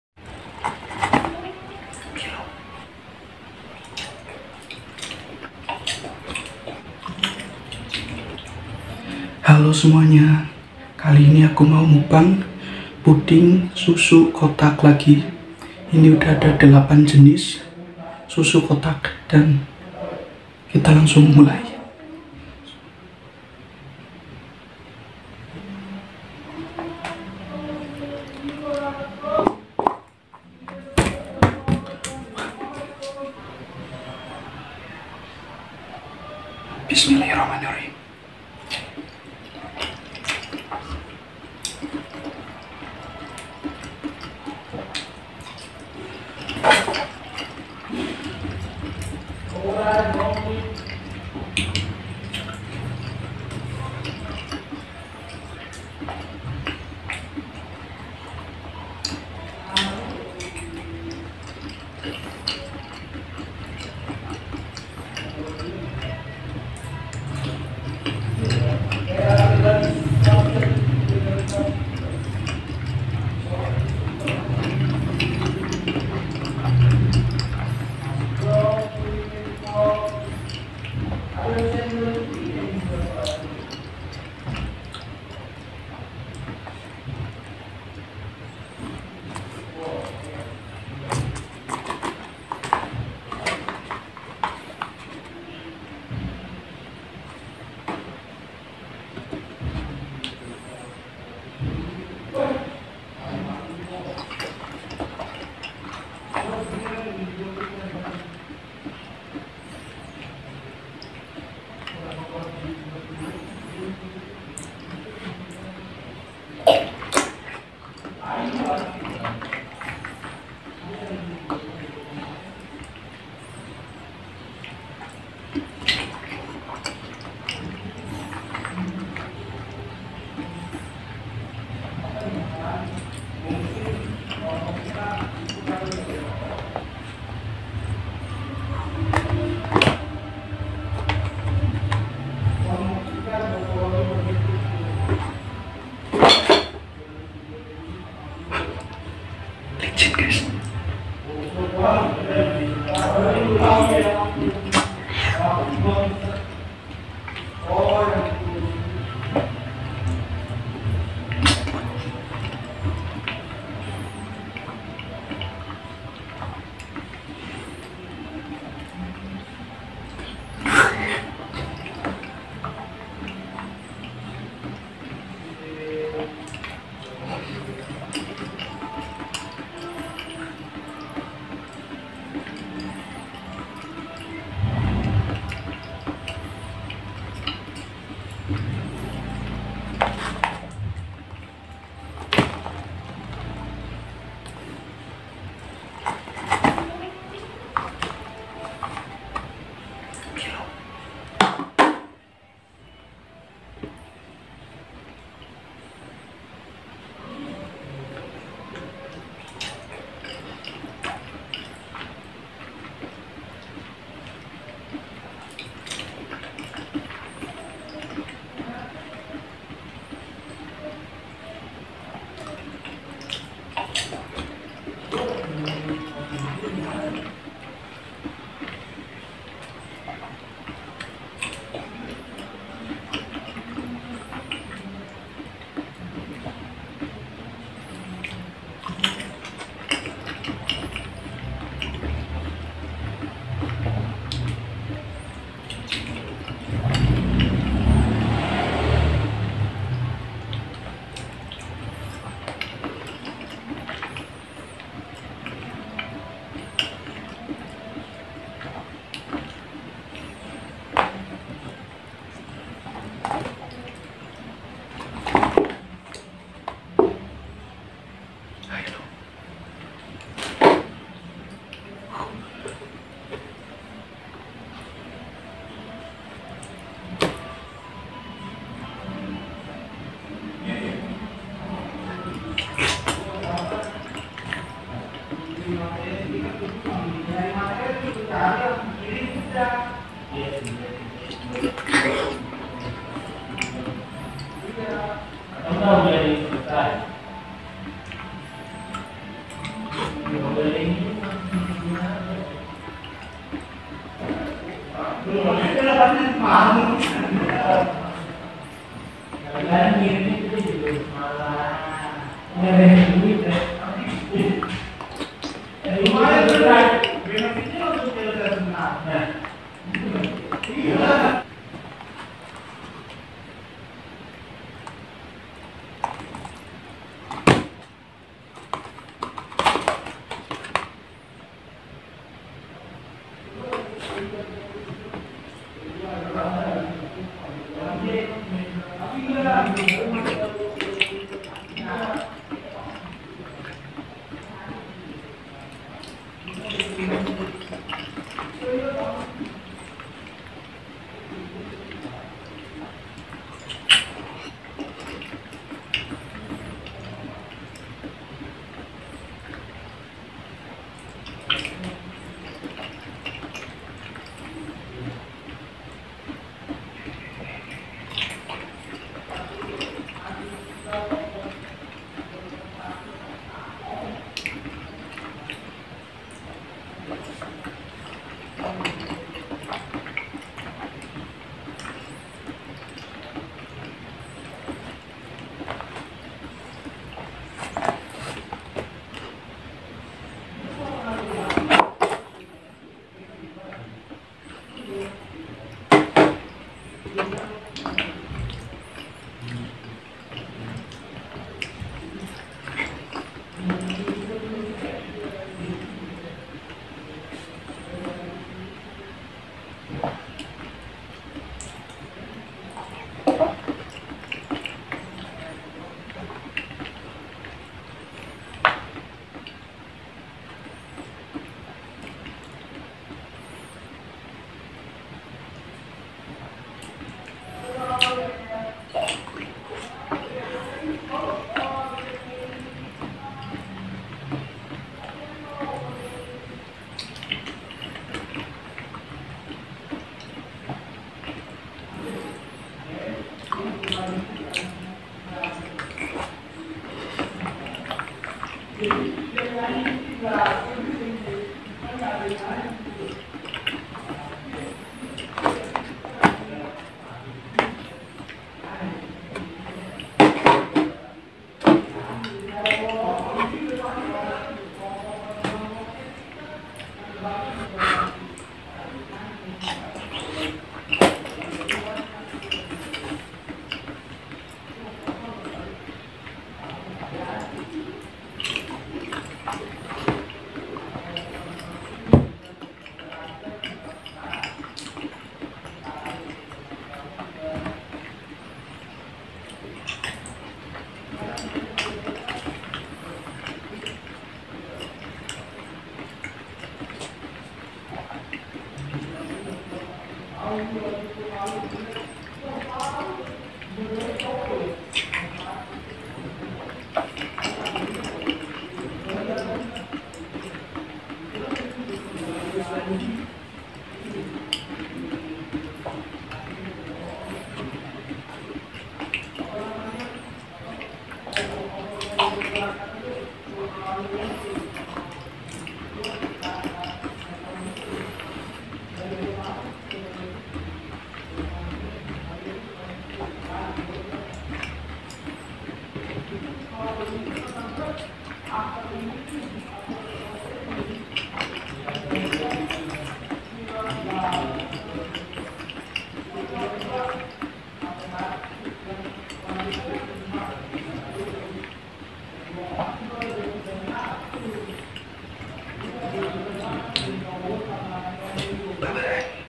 SUMPAH NAGIH!! MUKBANG PUDING SUSU KOTAK MILO, INDOMILK, TANGO, CHOCOLATOS, HILO, OVALTINE, CIMORY
Halo teman-teman semuanya kali ini aku mau Mukbang puding susu kotak lagi, aku udah buat 8 macam puding susu kotak nih yaitu ada puding susu kotak INDOMILK RASA pisang, puding susu kotak tango, puding susu kotak chocolatos, puding susu kotak Milo, puding susu kotak Hilo school, puding susu kotak ovaltine, puding susu kotak Cimory yogurt rasa coklat, dan puding susu kotak ultra milk rasa strawberry